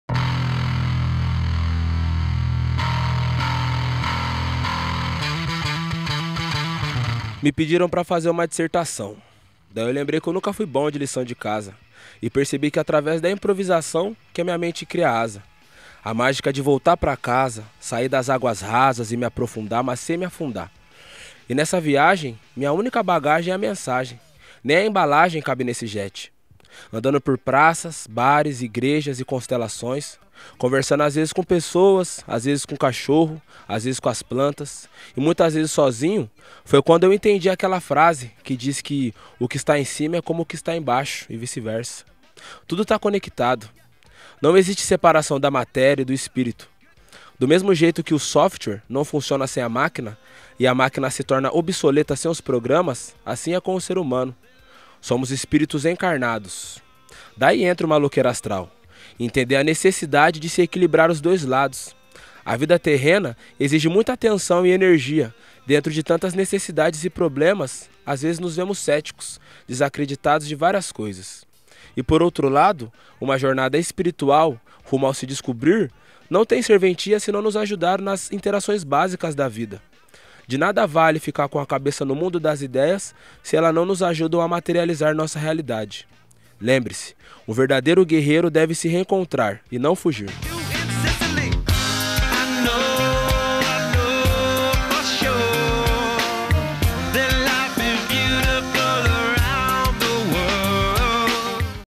Dissertação | Nego Max (Maloqueiro Astral)
No quadro Dissertação, a gente dá um tema e o rapper desenrola. Dessa vez, Nego Max escreveu um textinho sobre o matrêro conceito de Maloqueiro Astral.

O Fino da Zica é aquele canal do Youtube que toda segunda-feira posta um novo vídeo sobre um rapper convidado.